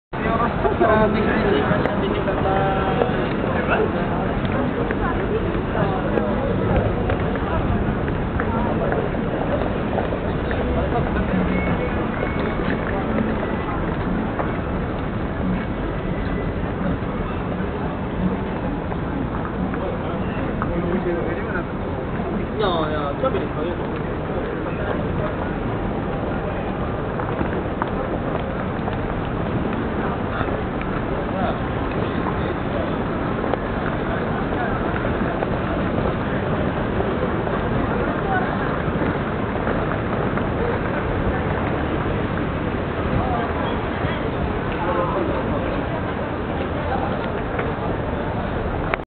"Shinjuku West Side Smoke Area" (100921Tu-1909)
Walking past the smoke area on the west side of Shinjuku Station.

新宿駅西口隣の煙ゾーン  東京